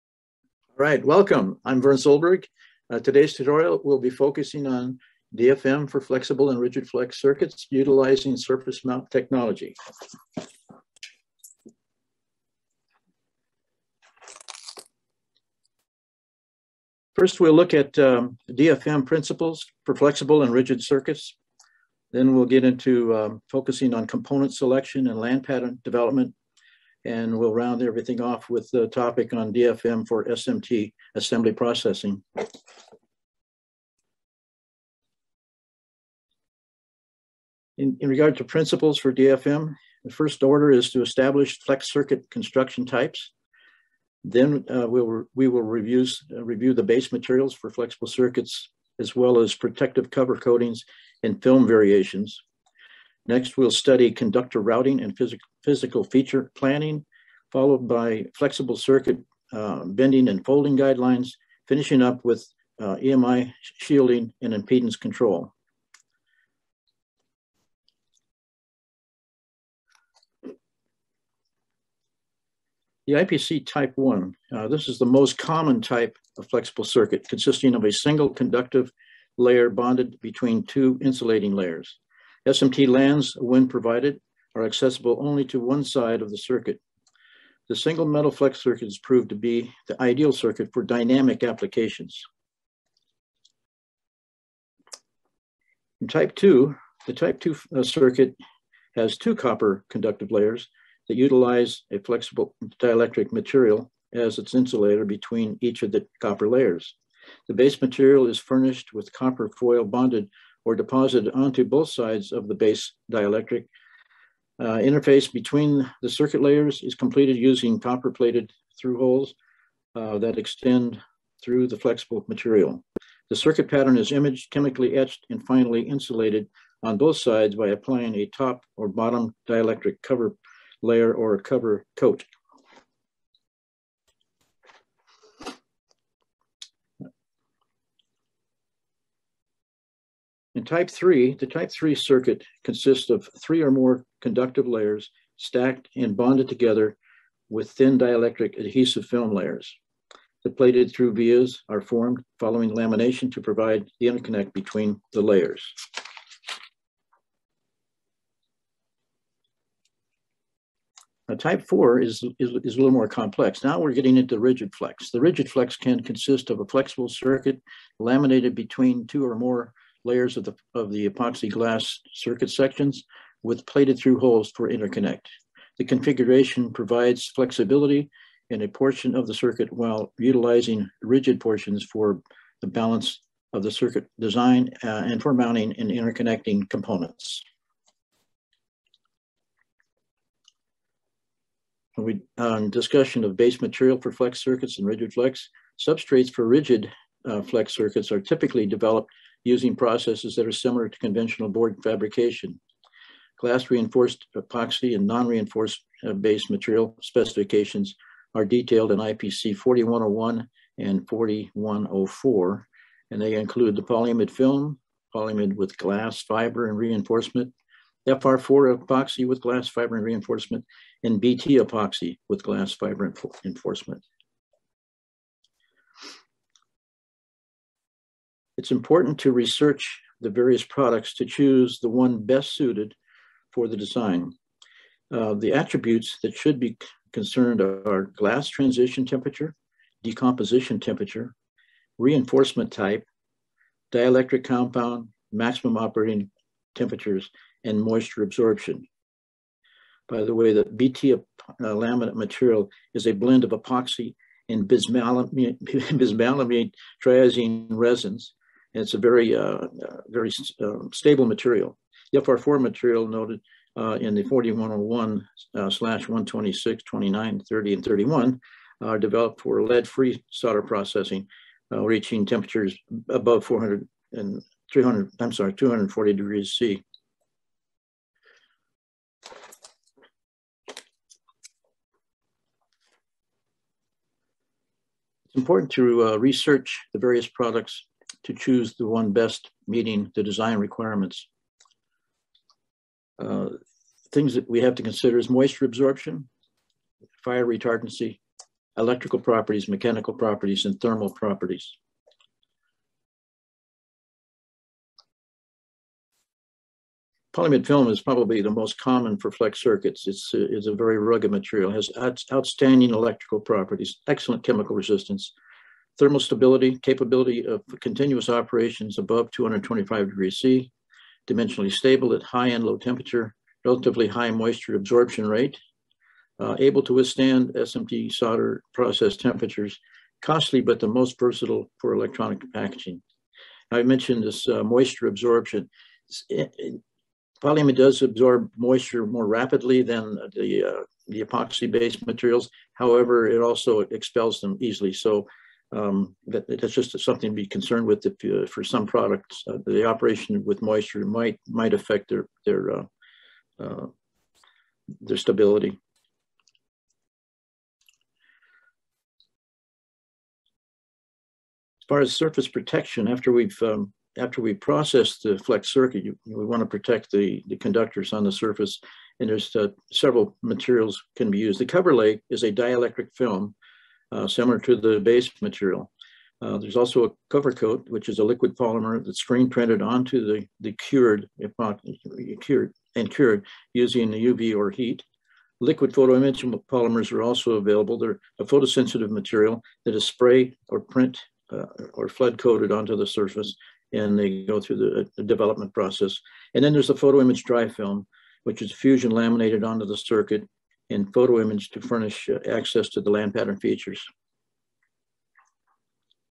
DFM for Flex and Rigid-Flex Utilizing SMT | Sierra Circuits
Developing a flexible circuit for surface mount component assembly entails a unique set of requirements. To quote a leading flexible circuit fabricator “Understanding how these requirements interact will allow the PCB designer to create a flex circuit that balances both electrical and mechanical features into a reliable, cost effective interconnect solution”. There are a broad number of reasons for developing the flexible and rigid-flex circuit.

• Increased wiring density- flexible circuits are capable of providing very narrow
conductors requiring only 25% of the space typically reserved for hard-wire
interconnect.
• SMT component interconnect- Land pattern features can be incorporated within the
flexible circuit pattern to accommodate the mounting and interconnecting both
passive and active surface mount components.
• Mechanical compliance- the thin base material is not only flexible but can be
shaped to fit unique and often complex 3D interconnect applications and enables
the inclusion of rigid materials when greater physical support is warranted.

Topics of discussion:

1. DfM principles for flexible and rigid-flex circuits

Establishing flex circuit construction types
Base materials for flexible and rigid flex
Cover coating and film variations
Conductor routing and physical feature planning
Flexible circuit bending and folding guidelines
EMI shielding and impedance control methodology

2. SMT component selection and land pattern development

Passive R and C devices
SOICs
QFN and QFP
BGA and CSP

3. DfM for SMT assembly processing

SMT assembly process overview
Preparation for SMT assembly
Land pattern reinforcement
Conductor to land interface
SMT component area support